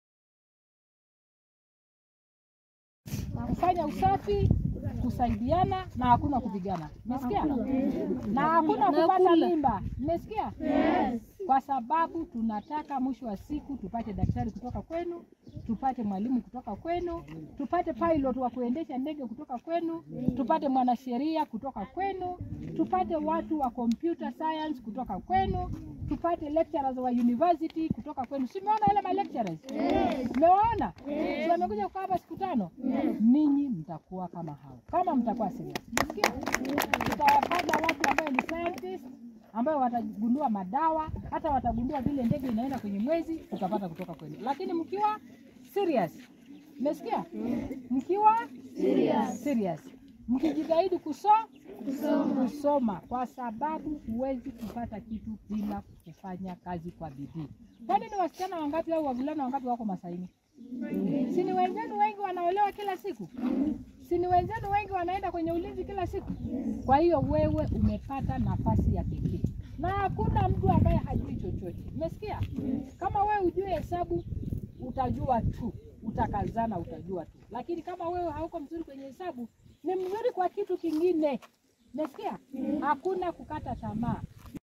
Maanda inspires ESS learners
We believe that when young indigenous pastoralist learners interact with role models from their own community, they are inspired to achieve their dreams. PWC Executive Director took some time during her latest visit to Emanyata Secondary School to engage with the learners and inspire them to be the best they can be by focusing on their studies.